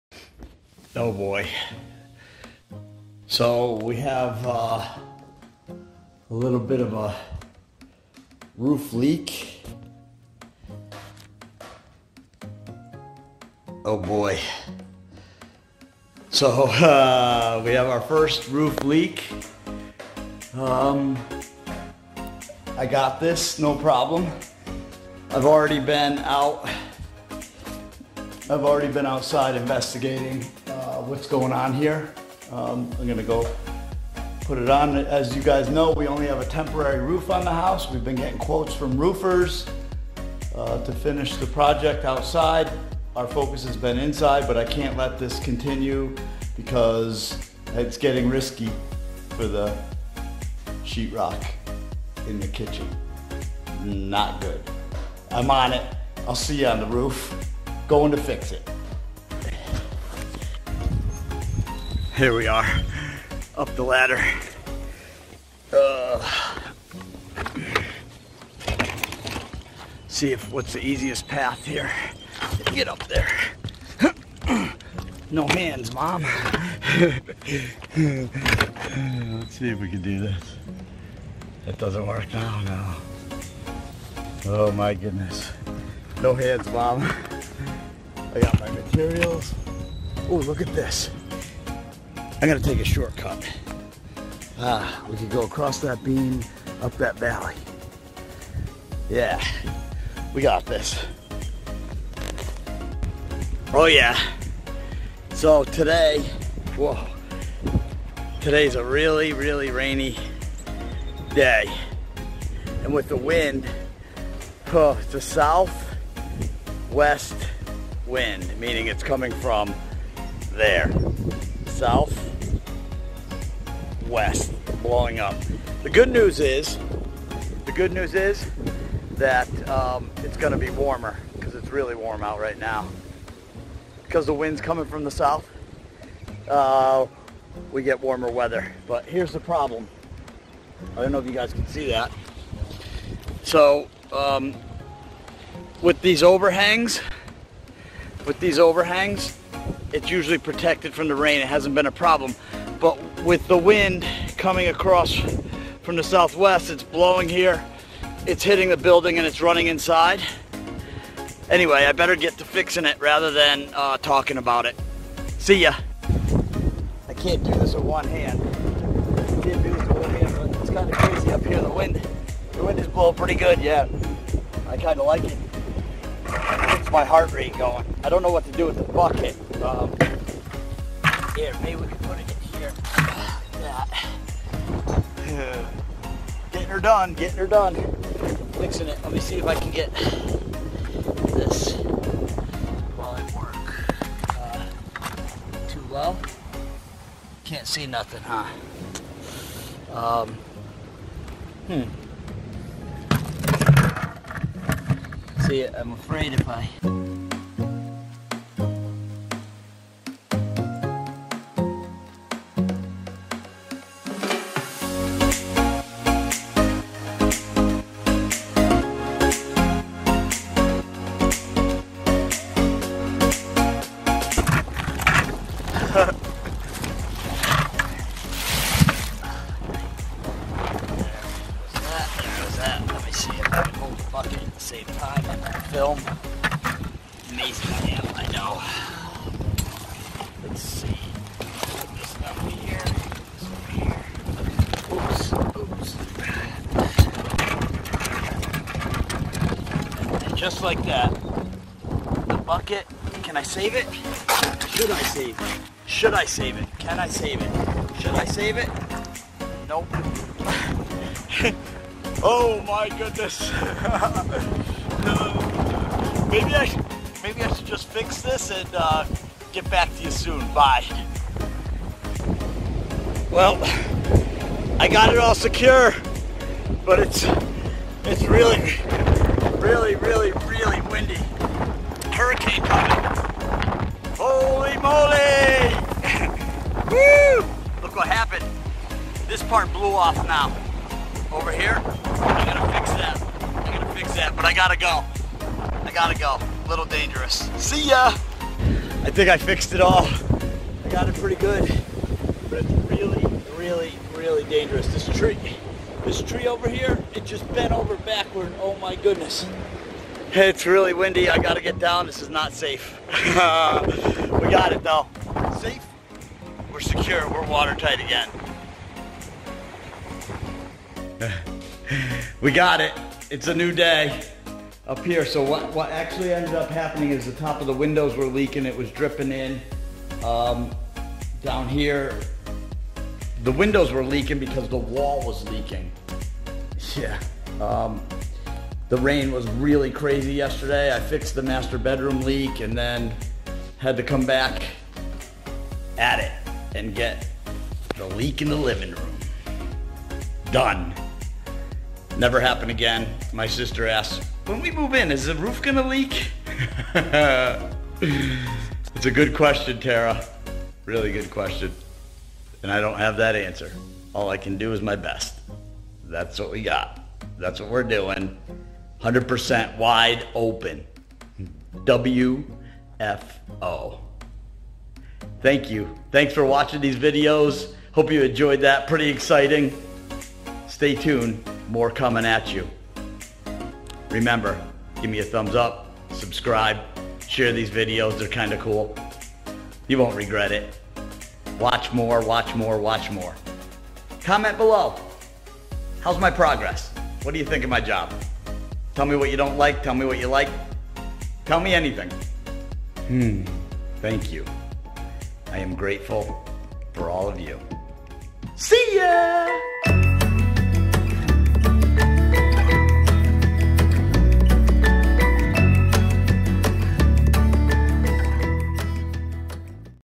TARA'S NEW HOME | Special Episode ( ROOF LEAK FIXED! DONE! ) | TonyB
This week will be different, starting today  I will show you the progress that I am making renovating my little sister's new home. Join me as we make changes, destroy and create in this beautiful home.